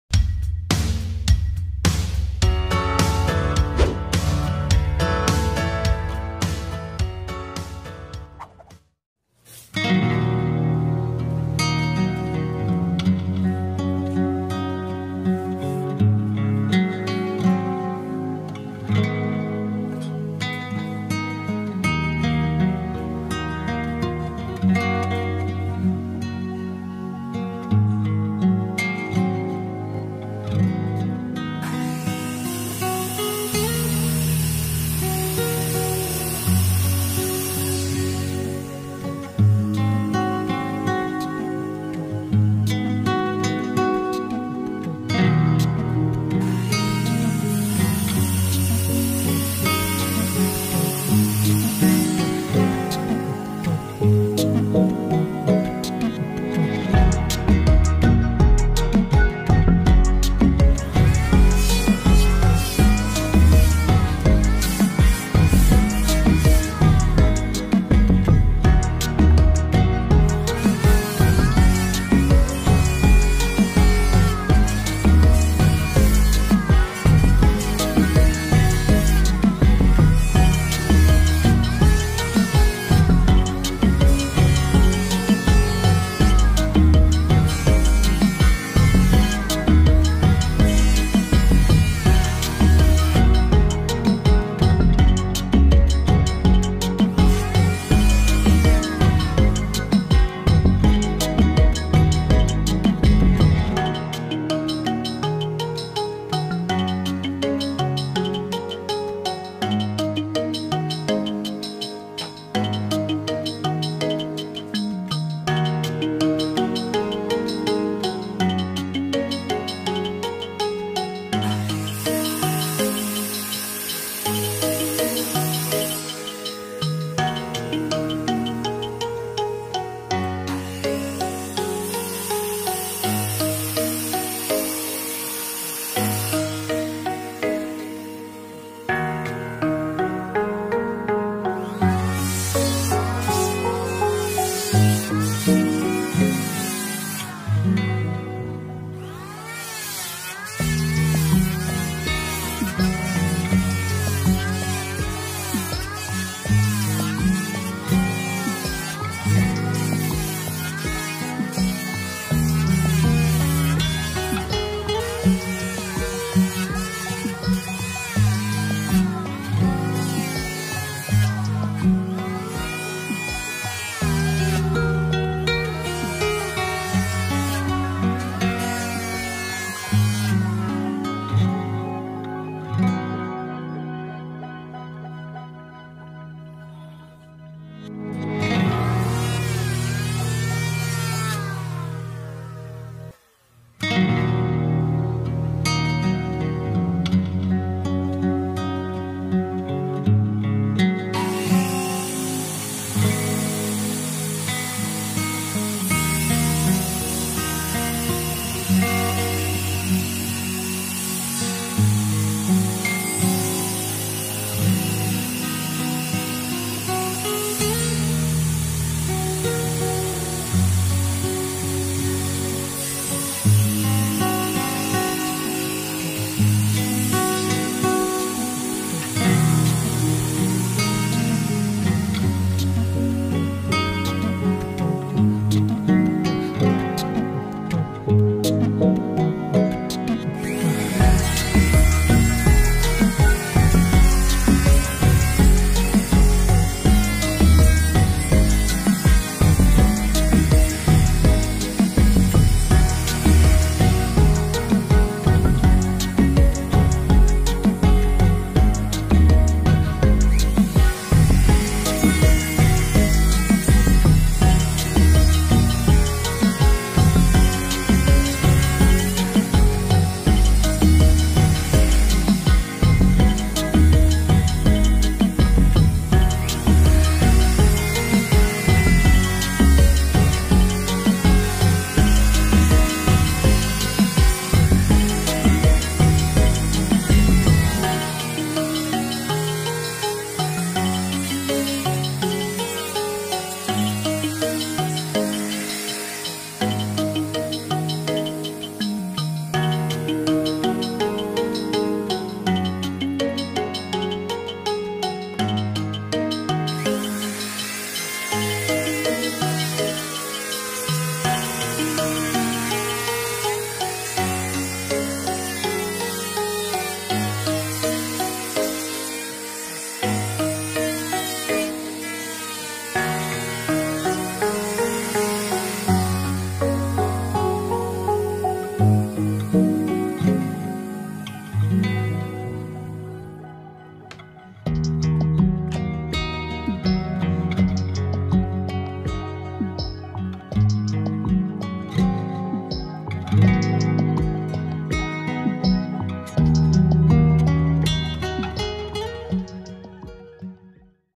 Recomendasi Daun Pintu Yang Mewah Dan Elegan Cocok Untuk Rumah Impian Anda
Berikut adalah beberapa recomendasi daun pintu rumah yang mewah. Dan juga di sertai layer video beberapa cara penyerutan juga profilan dan penggergajian.